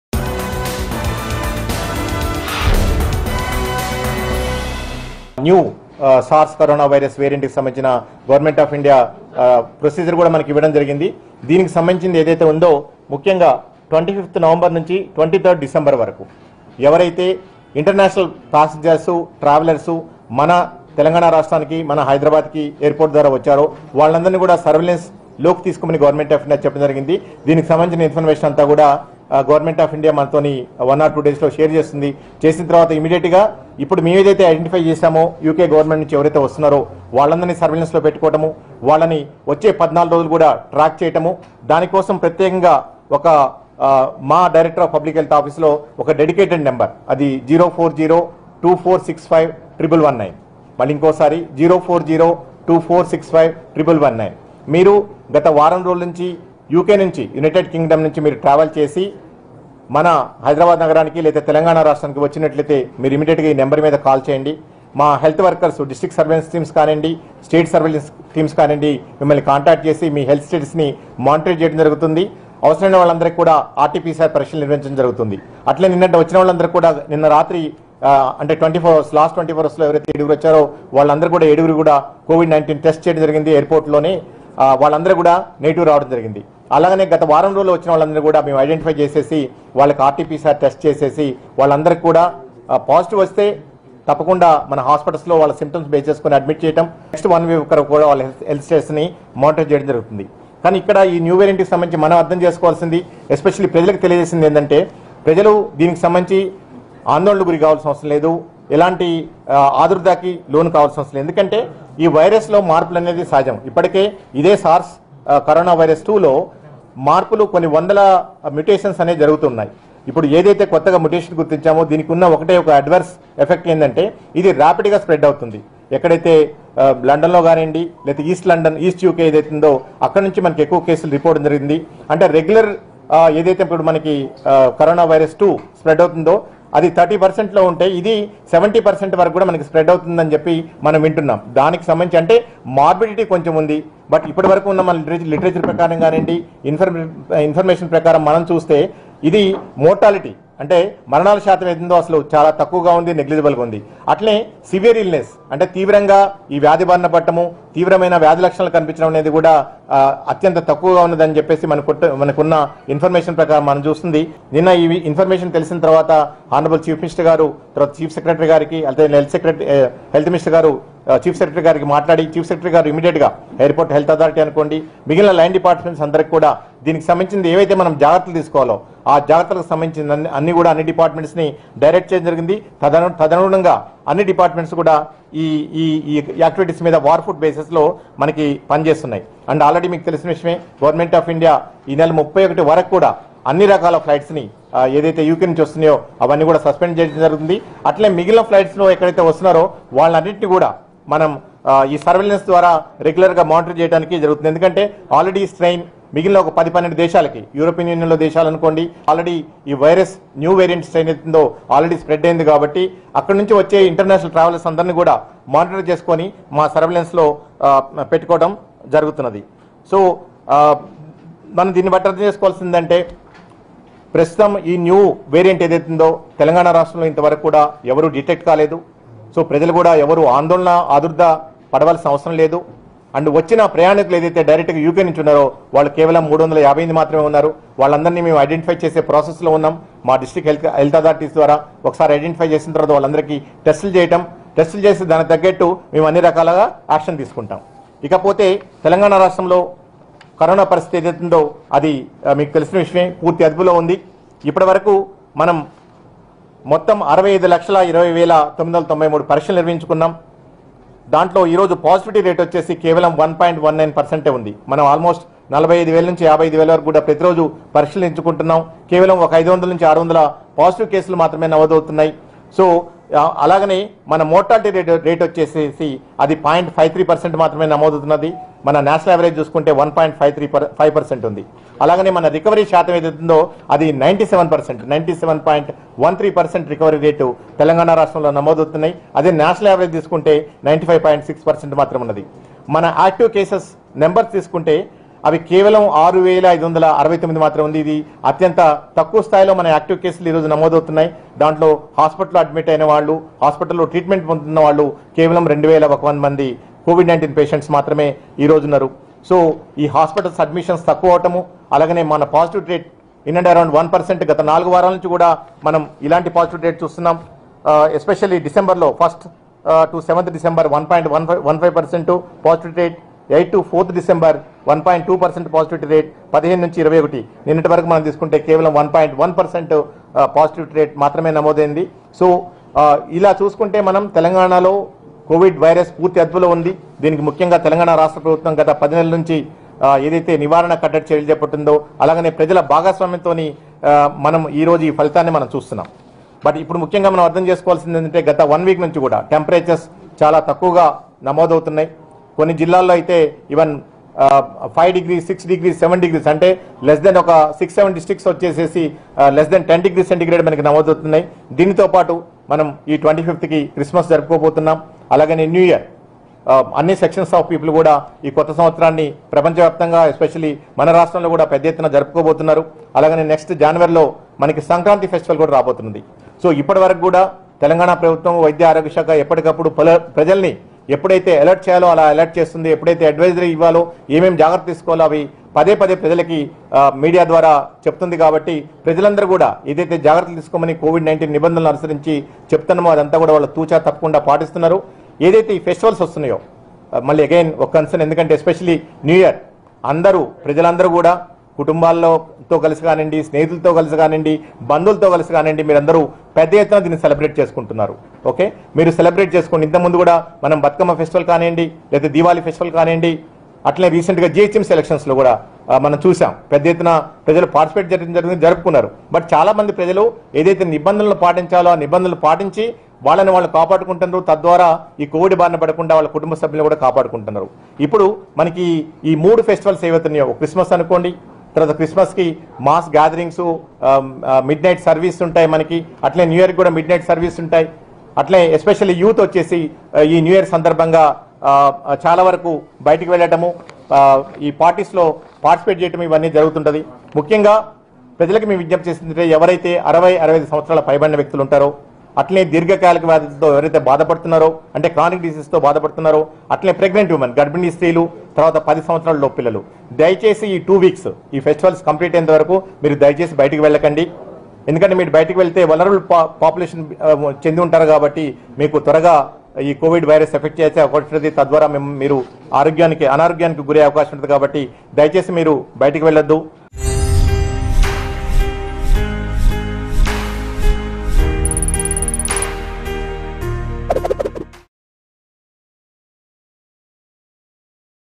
Telangana Health Director Srinivasa Rao About New COVID-19 Strain In UK | CoronaVirus | Mango News
Telangana Health Director Srinivasa Rao About New COVID-19 Strain In UK.